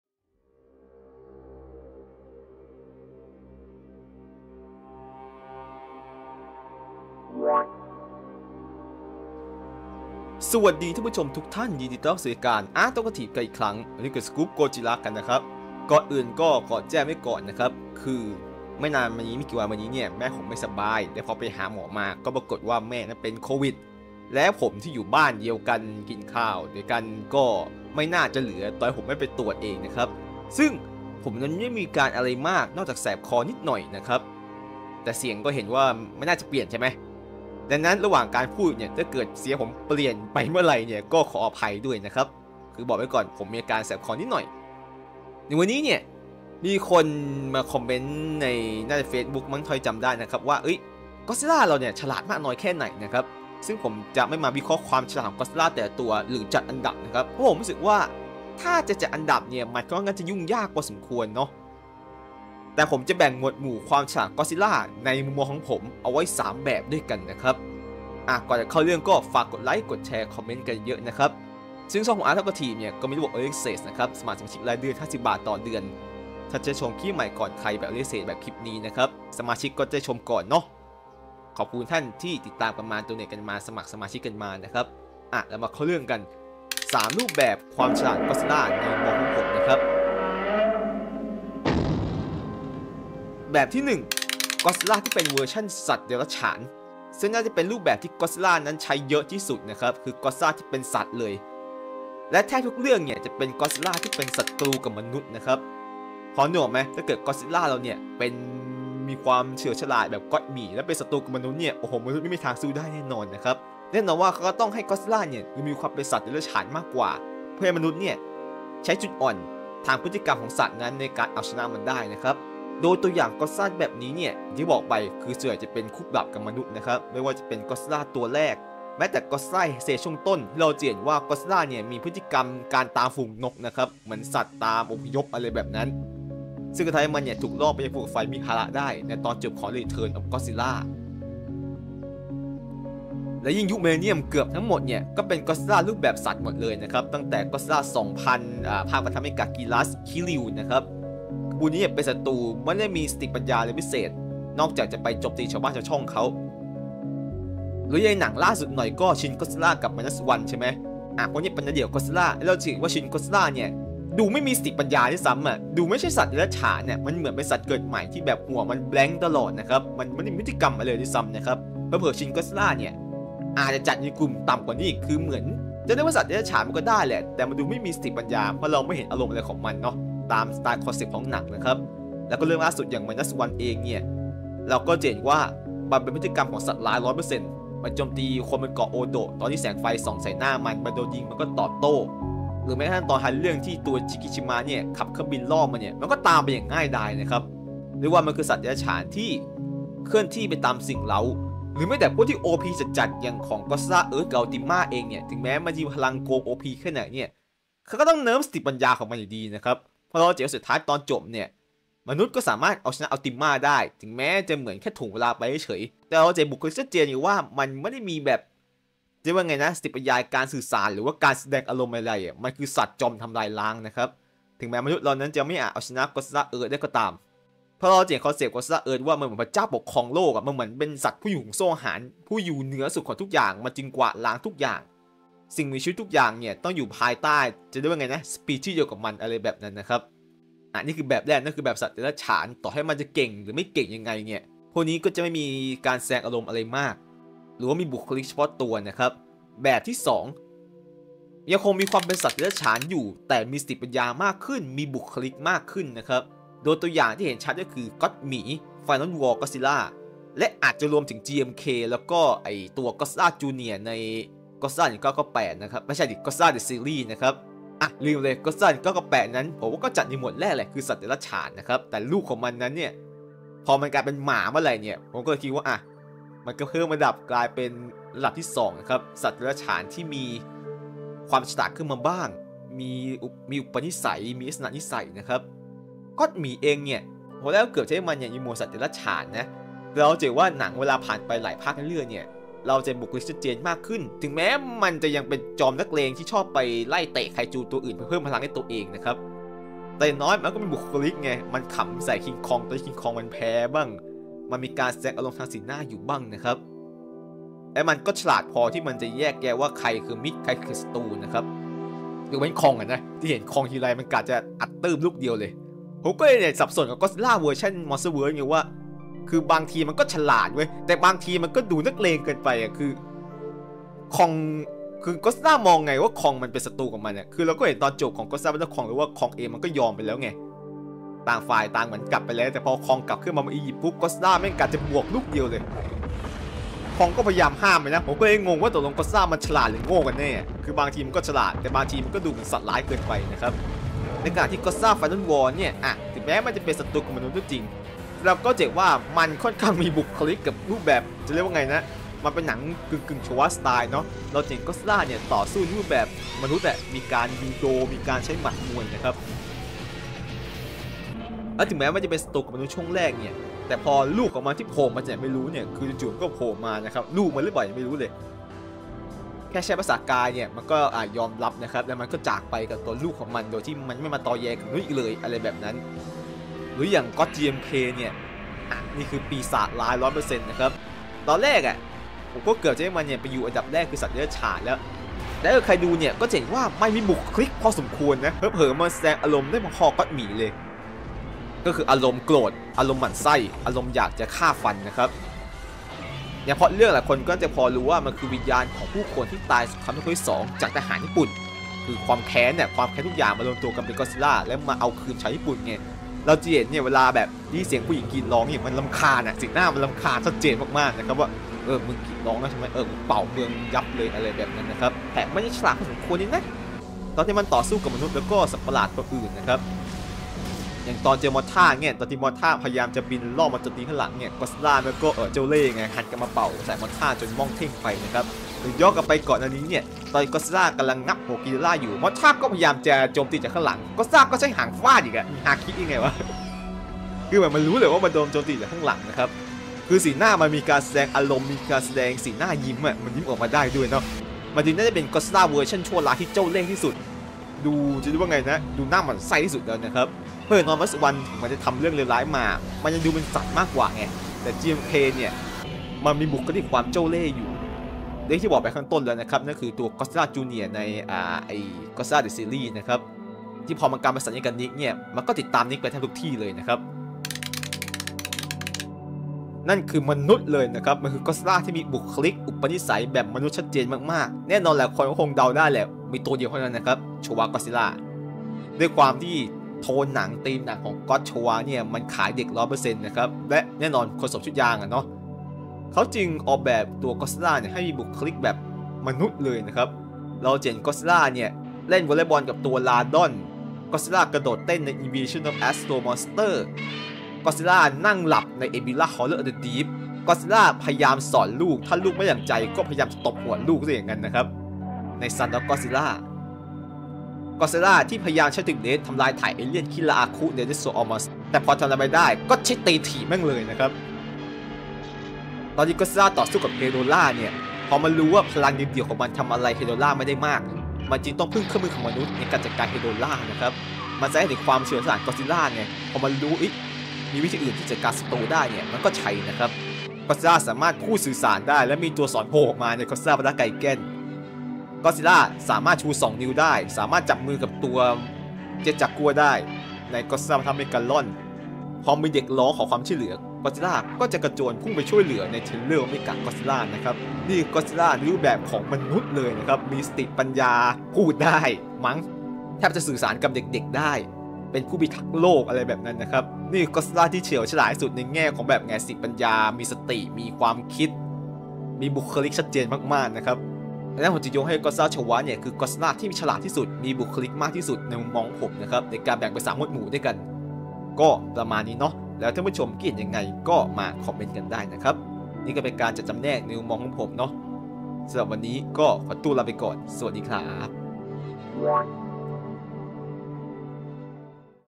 Godzilla ตัวไหนฉลาดที่สุด?
ในคลิปนี้จะพูดถึงก็อตซิลล่า โดยแบ่งจำแนกระดับสติปัญญาออกไป 3 แบบ